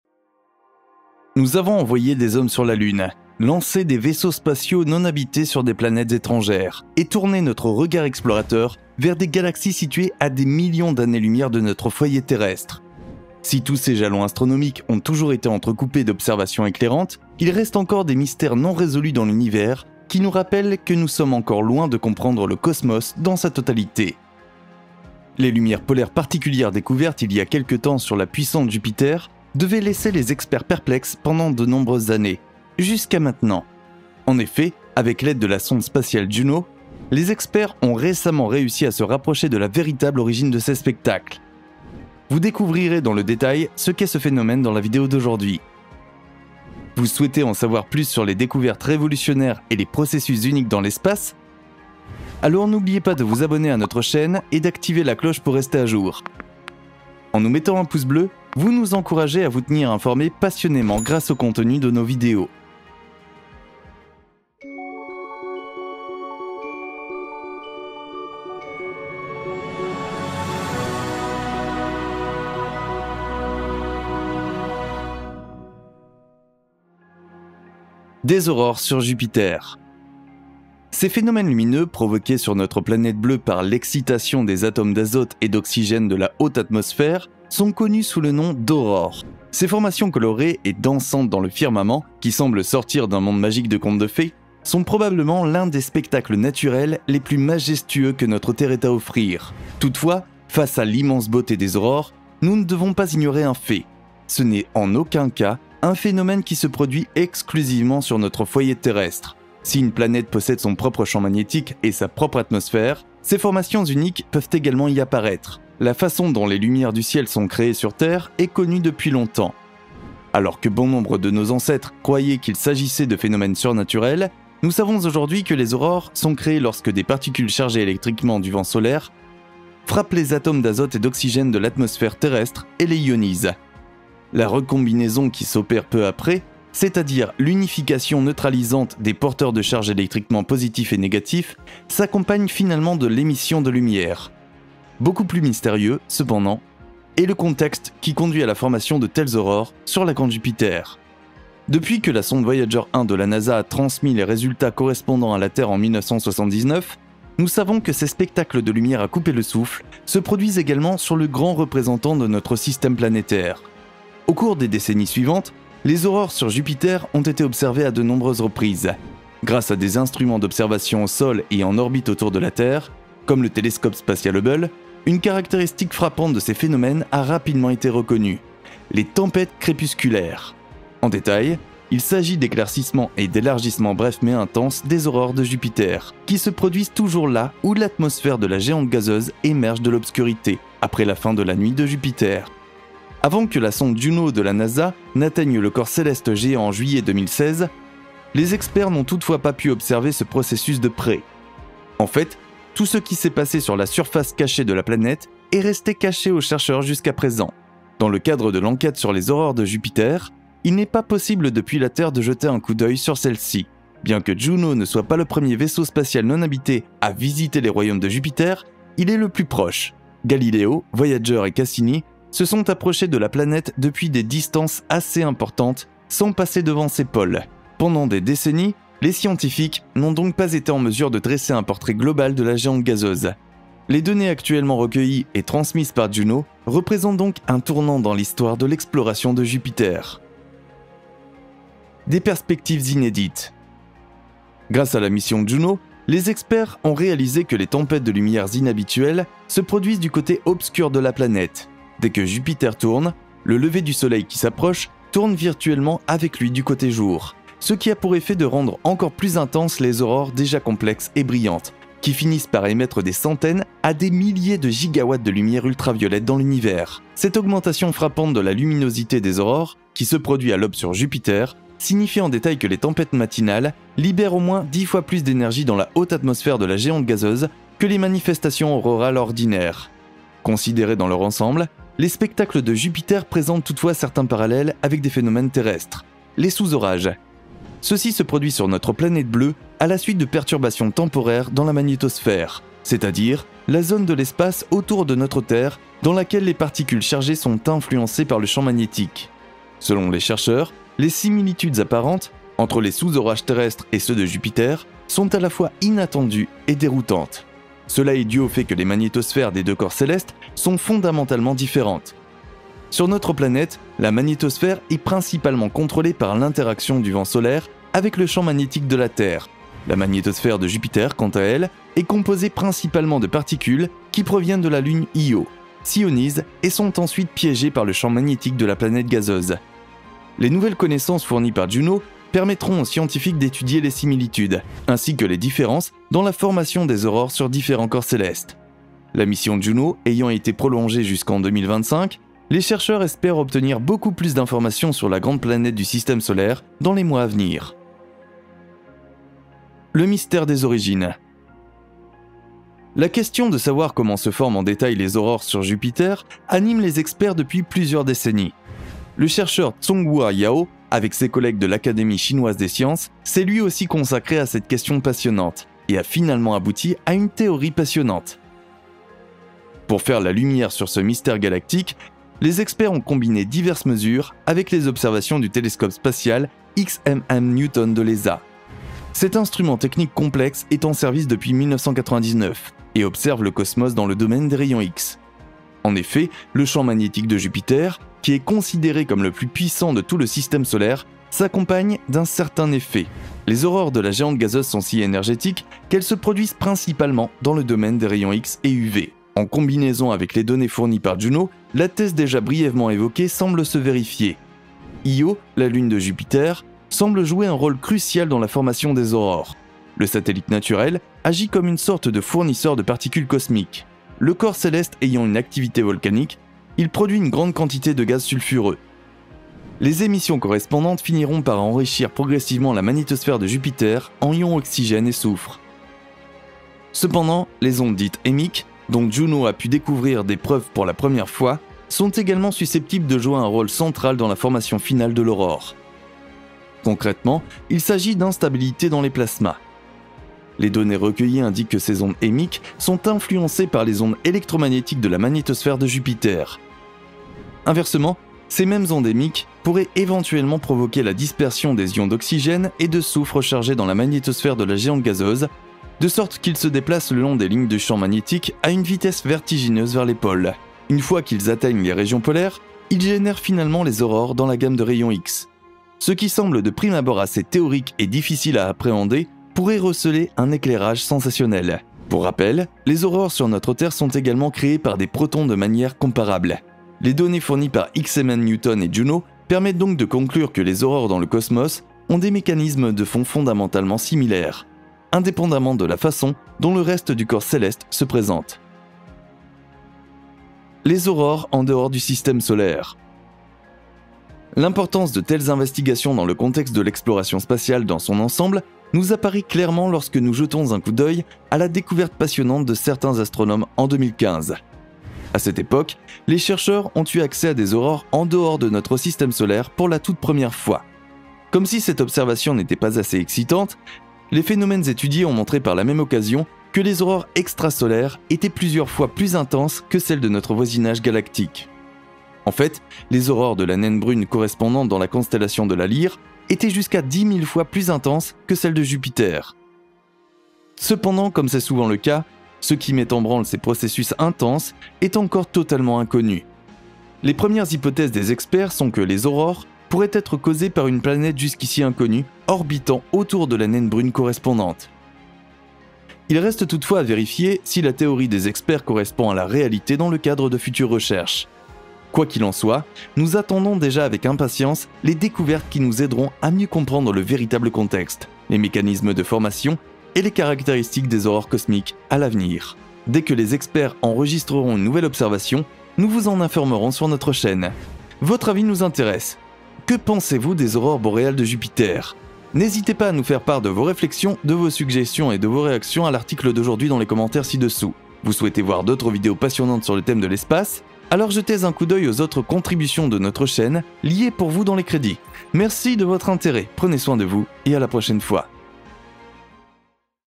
La NASA révèle les origines obscures des lumières de Jupiter !
Nous avons envoyé des hommes sur la lune, lancé des vaisseaux spatiaux non habités sur des planètes étrangères et tourné notre regard explorateur vers des galaxies situées à des millions d'années-lumière de notre foyer terrestre. Si tous ces jalons astronomiques ont toujours été entrecoupés d'observations éclairantes, il reste encore des mystères non résolus dans l'univers, qui nous rappellent que nous sommes encore loin de comprendre le cosmos dans sa totalité. Les lumières polaires particulières découvertes il y a quelque temps sur la puissante Jupiter devaient laisser les experts perplexes pendant de nombreuses années - jusqu'à maintenant ! En effet, avec l'aide de la sonde spatiale Juno, les experts ont récemment réussi à se rapprocher de la véritable origine de ces spectacles. Vous découvrirez dans le détail ce qu'est ce phénomène dans la vidéo d'aujourd'hui !